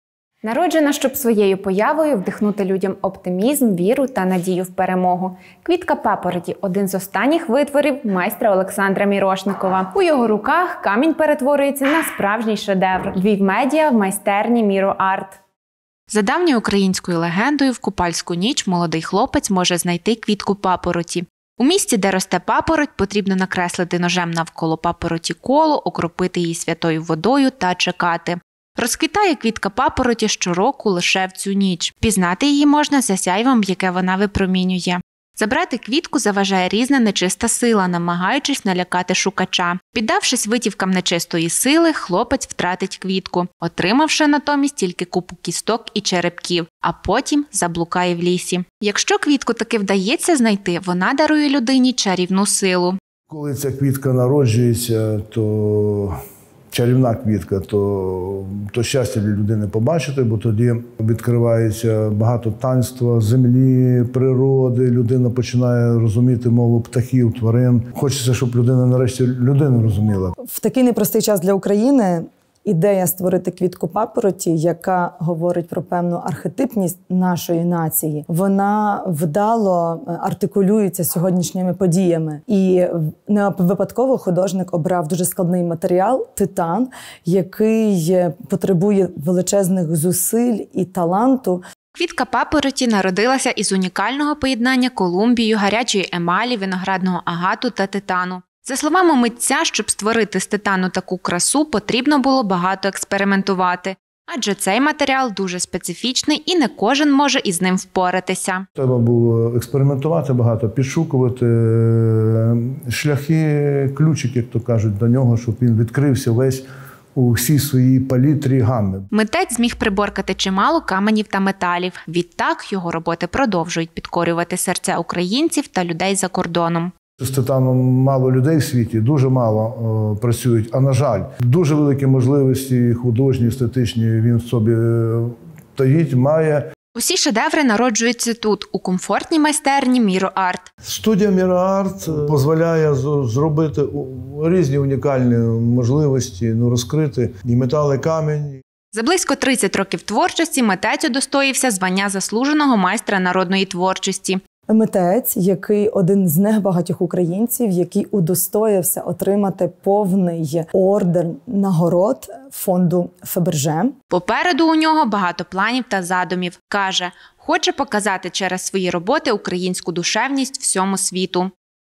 Квітка папороті: геніальний витвір Олександра Мірошникова
Квітка папороті - один із останніх витворів майстра Олександра Мірошникова.

Митець створив цю квітку із унікального поєднання колумбію, гарячої емалі, виноградного агату та титану.

За близько 30 років творчості він удостоївся звання заслуженого майстра народної творчості. Також він повний кавалер нагород меморіального фонду Фаберже.

Деталі — дивіться у сюжеті LVIV.MEDIA.